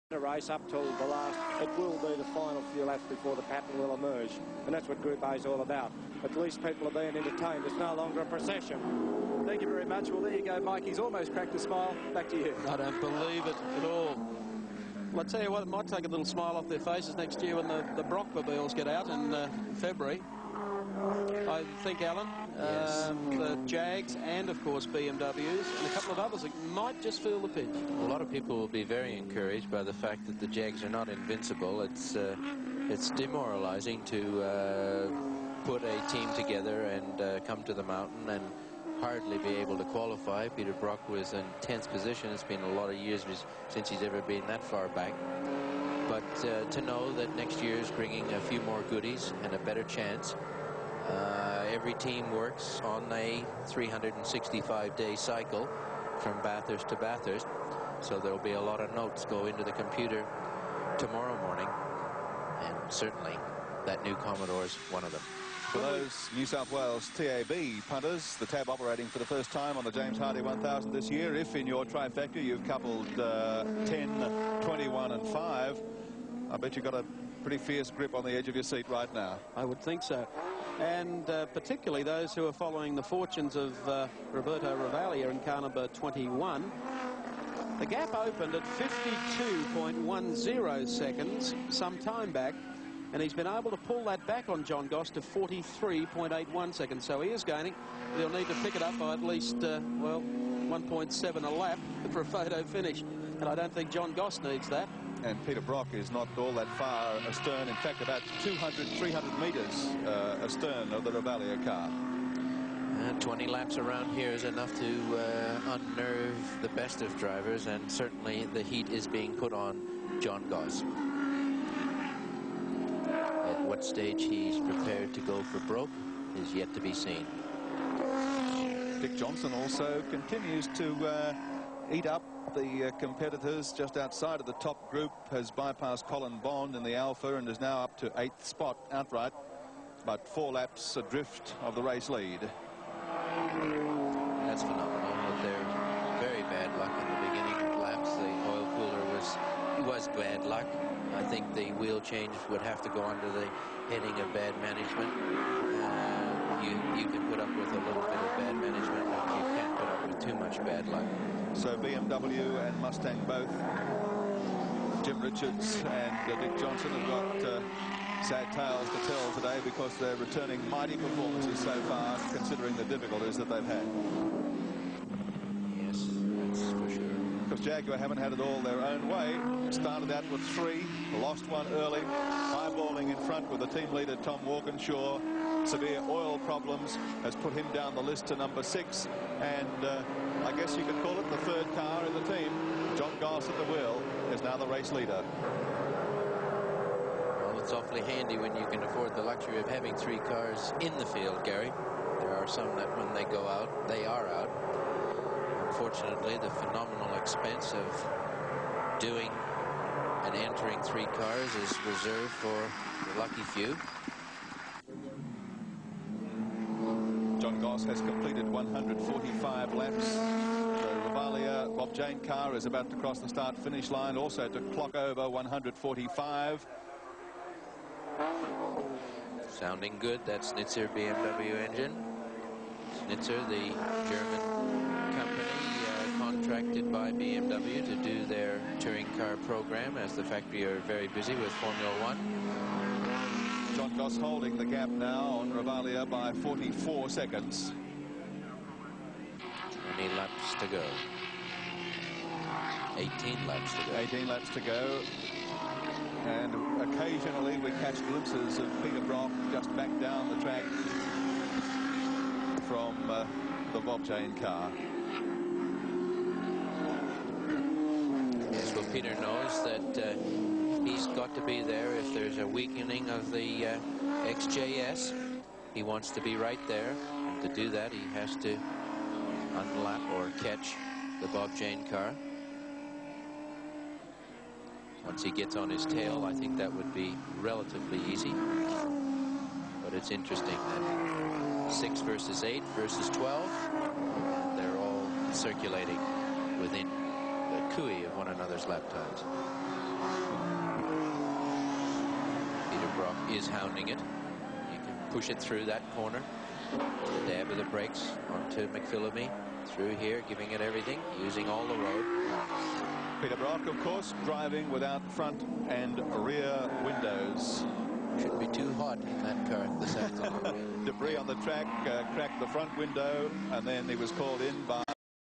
Roberto Ravaglia @ Bathurst. Bob Jane T-Mart BMW 635csi
Classic Touring Car Action. From 1985 with Roberto Ravaglia playing catch up at Bathurst in the Bob Jane T-Mart BMW 635csi